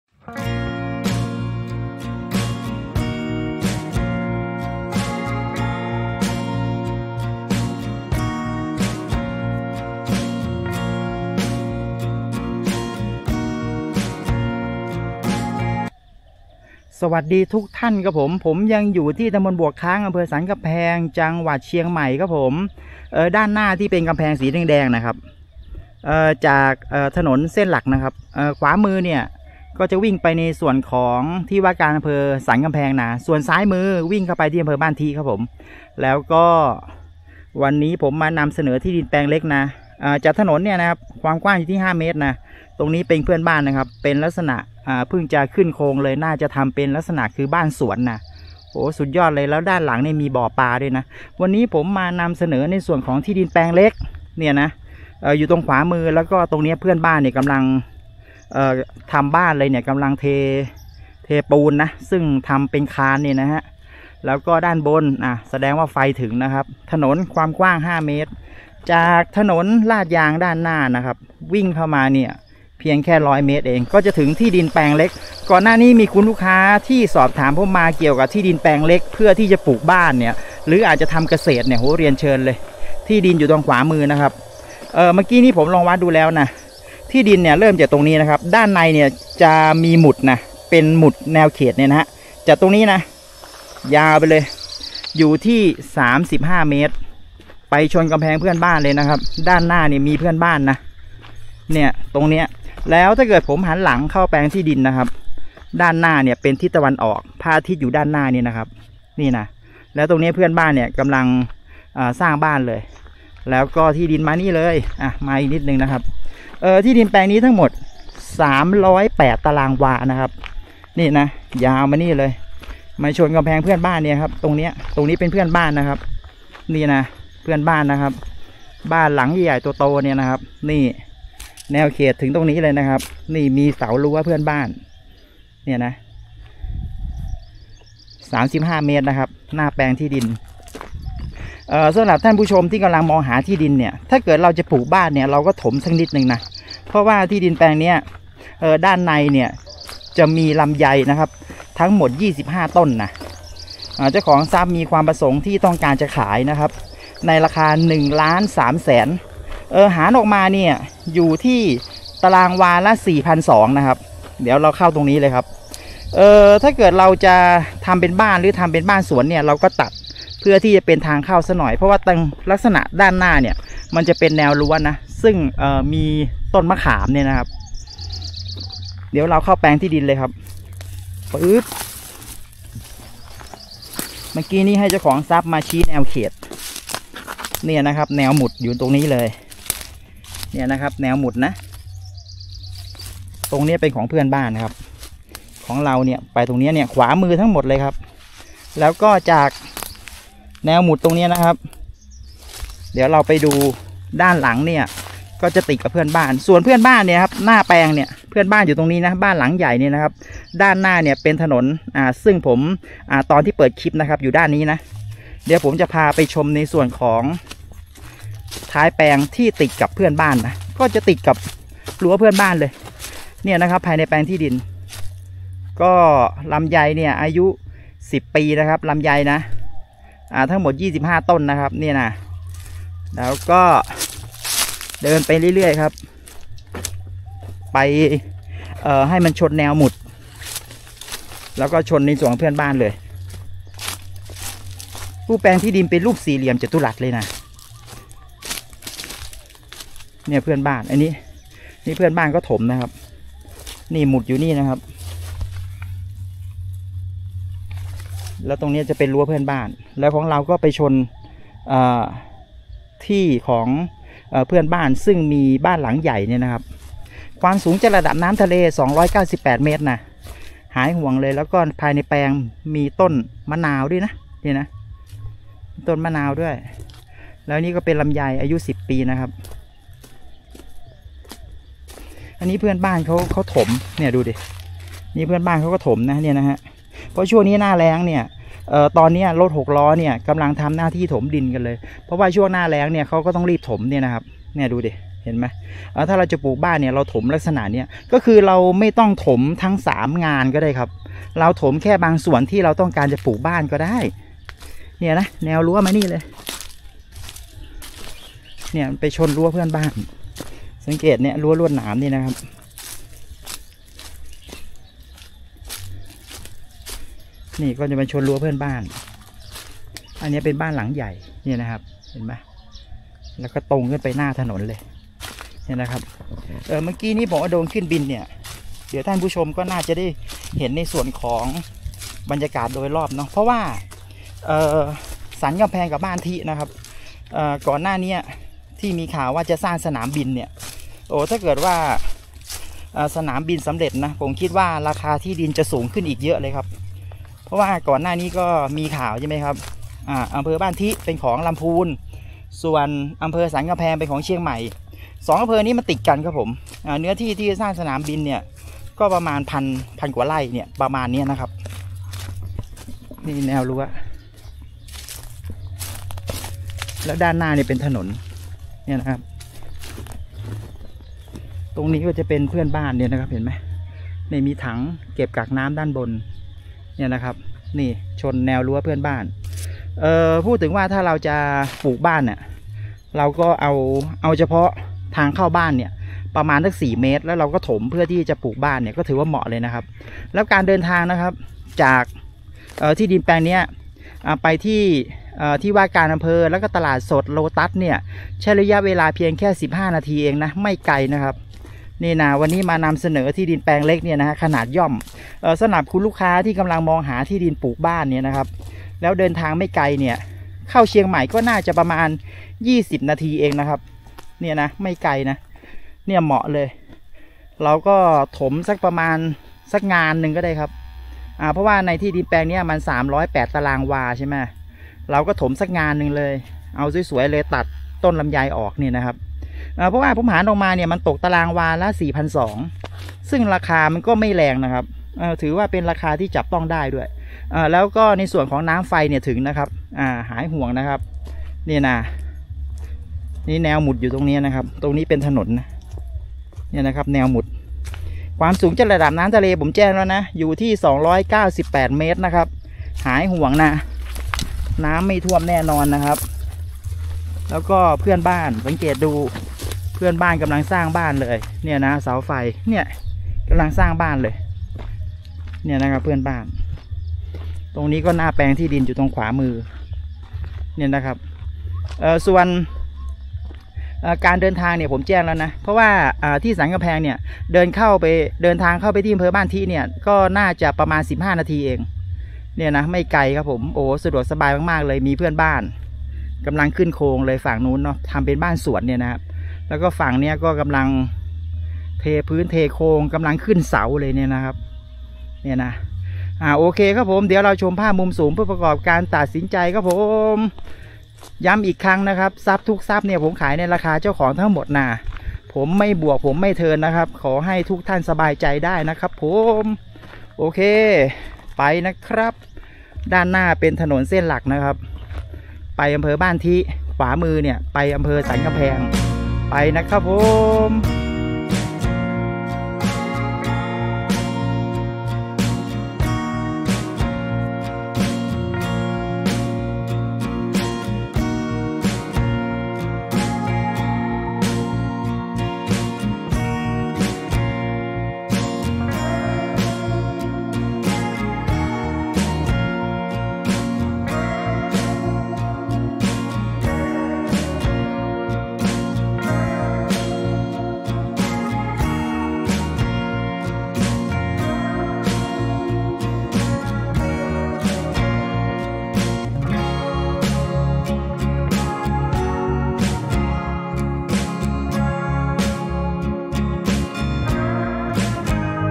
ที่ดินเปล่าEP629.ที่ดินแปลงเล็ก308ตรว.ราคา1.3ล้านพิกัดต.บวกค้าง/อ.สันกำแพง/เชียงใหม่​
#​น้ำไฟถึง, มี​เพื่อนบ้าน​ไม่​เปลี่ยว
#​เหมาะปลูก​บ้าน, เดินทางสะดวก
# โฉนด​พร้อม​โอน